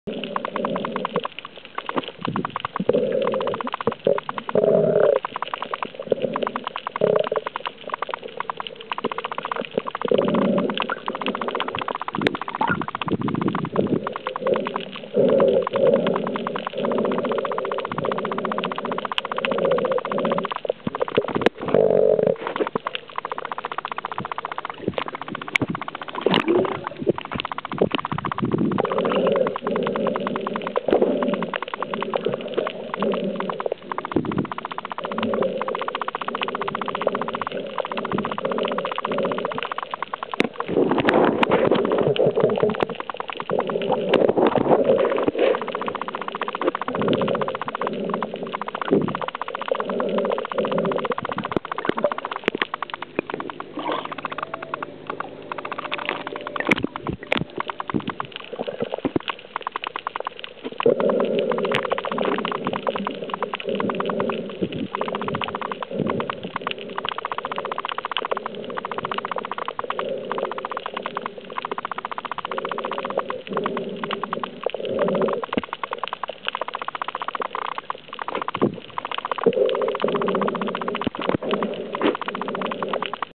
Schnorcheln Mango Bay Koh Tao2
Thailandurlaub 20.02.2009 bis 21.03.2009 Roman, Christian und Max